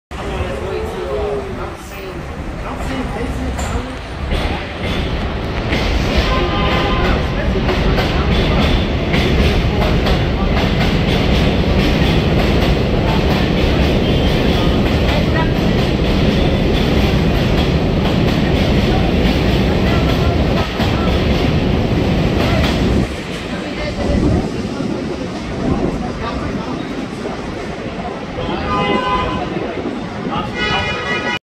Transit Fanning #310: (B)est (T)rain (E)ver
I caught the yellow [B] easter egg on the [N] line at the Astoria blvd with my family

Taken on April 9th 2021 at 2:36pm

All ocs, music themes, videos, & sound effects belongs to their respectful owners